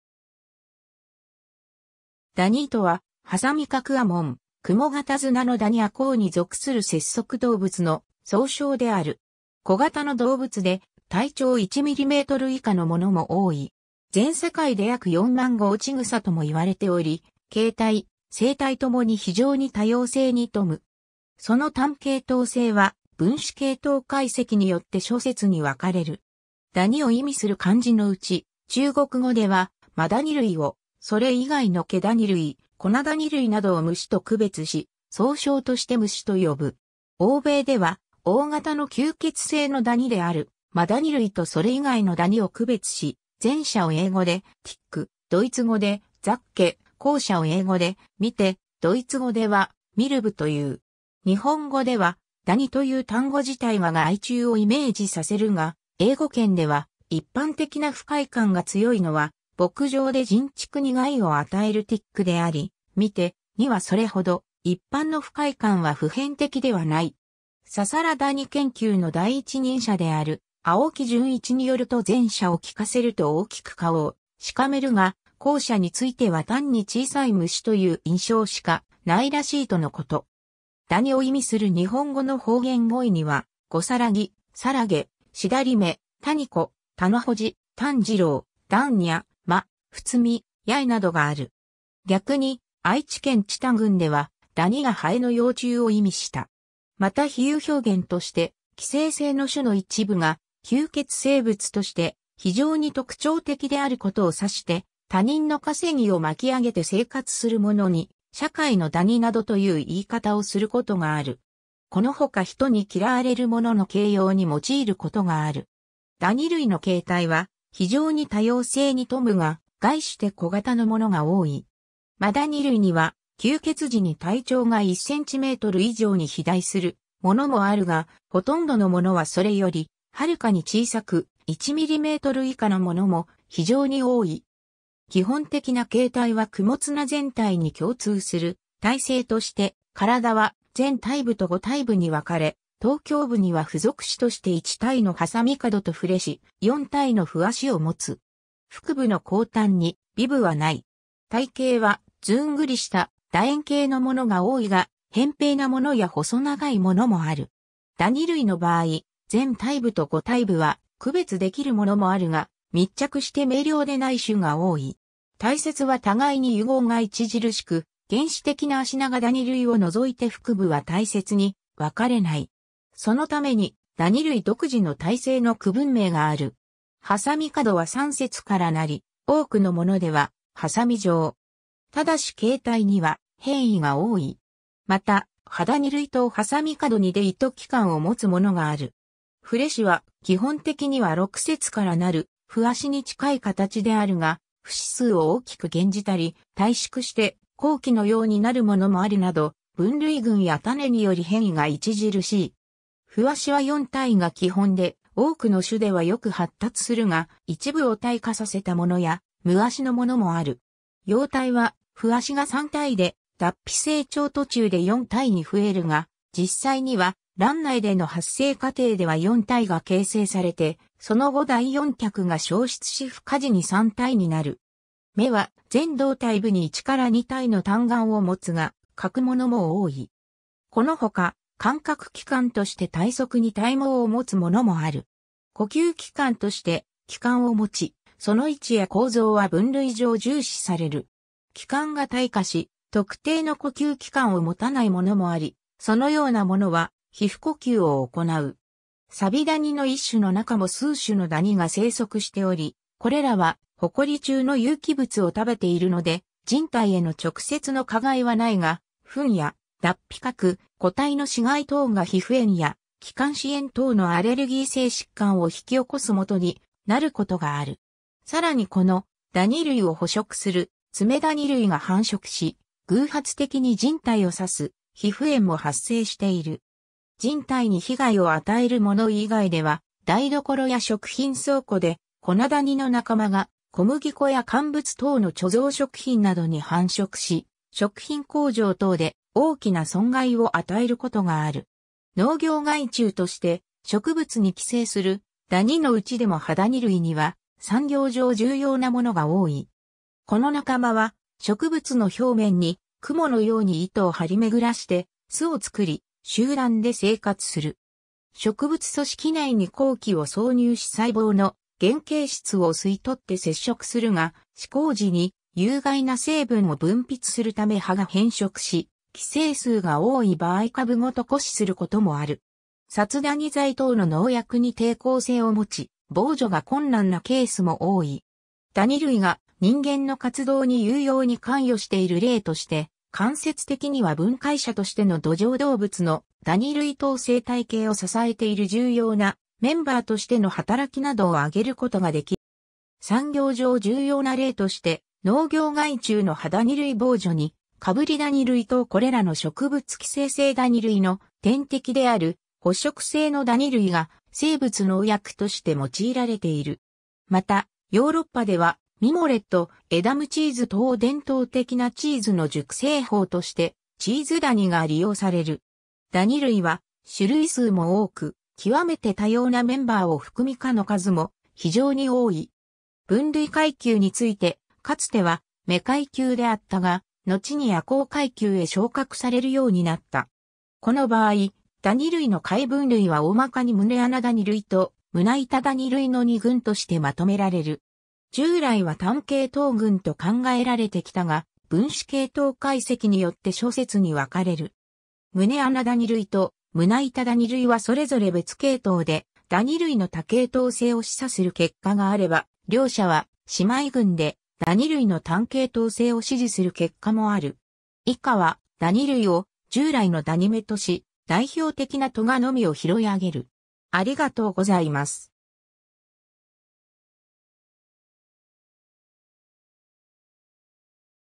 ダニ
ダニ（ダニ類、壁蝨、蜱、蟎、螕）とは、鋏角亜門クモガタ綱のダニ亜綱（またはダニ目、学名：Acari）に属する節足動物の総称である。
小型の動物で、体長1mm以下のものも多い。
 全世界で約4万5千種とも言われており、形態・生態ともに非常に多様性に富む。
その単系統性は分子系統解析によって諸説に分かれる。
 ダニを意味する漢字の内、中国語ではマダニ類を蜱（pí）、それ以外のケダニ類、コナダニ類などを蟎（mǎn）と区別し、総称として蜱蟎と呼ぶ。
 欧米では大型の吸血性のダニであるマダニ類とそれ以外のダニを区別し、前者を英語でTick、ドイツ語でZecke、後者を英語でMite、ドイツ語ではMilbeという。
日本語では「ダニ」という単語自体が害虫をイメージさせるが、英語圏では一般的な不快感が強いのは牧場で人畜に害を与える "Tick" であり、"Mite" にはそれほど一般の不快感は普遍的ではない。
 ササラダニ研究の第一人者である青木淳一によると前者を聞かせると大きく顔をしかめるが、後者については単に小さい虫という印象しかないらしいとのこと。
 ダニを意味する日本語の方言語彙には、ごさらぎ（和歌山県）、さらげ（熊本県）、しだりめ（東京都八丈島）、たにこ（京都府）、たのほじ（島根県）、たんじろう（新潟県中魚沼郡）、だんにゃま（鹿児島県）、ふつみ（山口県）、やえ（山口県）などがある。
逆に愛知県知多郡では、だにがハエの幼虫を意味した。
 また比喩表現として、寄生性の種の一部が吸血生物として非常に特徴的であることを指して、他人の稼ぎを巻き上げて生活するものに「社会のダニ」などという言い方をすることがある。
このほか人に嫌われる者の形容に用いることがある。
 ダニ類の形態は非常に多様性に富むが、概して小型のものが多い。
マダニ類には吸血時に体長が1cm以上に肥大するものもあるが、ほとんどのものはそれよりはるかに小さく、1mm以下のものも非常に多い。
 基本的な形態はクモ綱全体に共通する体制として、体は前体部（頭胸部）と後体部（腹部）に分かれ、頭胸部には付属肢として1対の鋏角と触肢、4対の歩脚をもつ。
 腹部の後端に尾部はない。
体型はずんぐりした楕円形のものが多いが、扁平なものや細長いものもある。
 ダニ類の場合、前体部と後体部は区別できるものもあるが、密着して明瞭でない種が多い。
体節は互いに癒合が著しく原始的なアシナガダニ類を除いて腹部は体節に分かれない（フシダニ類、ニキビダニ類等には一見体節に見える環節があるが、二次的なものである）。
そのためにダニ類独自の体制の区分名がある。
 鋏角は3節からなり、多くのものでは鋏状。
ただし形態には変異が多い。
またハダニ類等鋏角に出糸器官をもつものがある。
 触肢は基本的には6節からなる歩脚に近い形であるが、節数を大きく減じたり、退縮して口器のようになるものもあるなど分類群や種により変異が著しい。
 歩脚は4対が基本で、多くの種ではよく発達するが、一部を退化させたものや無脚のものもある。
幼体は歩脚が3対で脱皮成長途中で4対に増えるが、実際には卵内での発生過程では4対が形成されて、その後第4脚が消失し孵化時に3対になる。
眼は前胴体部に1-2対の単眼を持つが、欠くものも多い。
このほか感覚器官として体側に体毛をもつものもある。
 呼吸器官として気管を持ち、その位置や構造は分類上重視される。
気管が退化し特定の呼吸器官を持たないものもあり、そのようなものは皮膚呼吸を行う。
 サビダニの一種（Aceria anthocoptes 歩脚は2対、腹部に二次的な環節がある ネコに付着吸血し肥大したマダニの1種 ダニ類は熱帯から極域まで世界中に分布し、高山から低地、乾燥地から湿地、土壌中、水中、家屋内や貯蔵食品内等の人工的な環境、さらには植物や動物の体組織中まで様々な環境に適応して生息する。一般に体が微小であることから、様々な微環境に応じてそれぞれ異なる種が棲み分けており、分布が局在している種もあるがコスモポリタン種も多い。また、一部の水生ダニ類では成体が陸上で生活する等、成長段階により生息環境が変化する種もいる。 食性もそれぞれの種の生息する微環境に適応し、菌食性、腐食性、捕食性、植食性、寄生性等クモ形類としては非常に多様である。注目されることの多い寄生性の種についても、寄生生物として高度に適応しているものから、宿主に対する捕食に近いもの、宿主の体組織を一種の微環境として生息しているものまで段階は様々である。また寄生性のツツガムシ類...